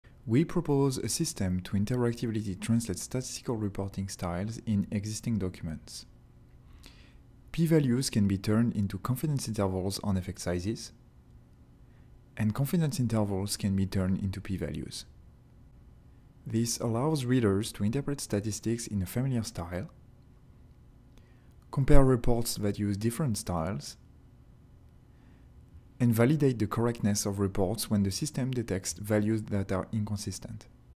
Statslator: Interactive Translation of NHST and Estimation Statistics Reporting Styles in Scienti...
Damien Masson, Sylvain Malacria, Géry Casiez, Daniel Vogel

UIST 2023: The ACM International Symposium on User Interface Software and Technology
Session: Data Dreamers: Math, Stats and Visualization

Inferential statistics are typically reported using p-values (NHST) or confidence intervals on effect sizes (estimation). This is done using a range of styles, but some readers have preferences about how statistics should be presented and others have limited familiarity with alternatives. We propose a system to interactively translate statistical reporting styles in existing documents, allowing readers to switch between interval estimates, p-values, and standardized effect sizes, all using textual and graphical reports that are dynamic and user customizable. Forty years of CHI papers are examined. Using only the information reported in scientific documents, equations are derived and validated on simulated datasets to show that conversions between p-values and confidence intervals are accurate. The system helps readers interpret statistics in a familiar style, compare reports that use different styles, and even validate the correctness of reports. Code and data

Video previews for Papers at UIST 2023